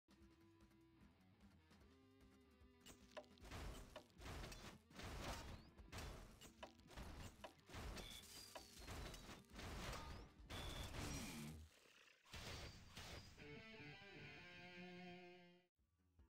World 2 - Level 8 (1.98)
This was a hell of a grind, but I'm happy to actually have sub2 for this. Frame perfect turn around after starting to move right saves like 3-4 frames over turning left at the start of the level.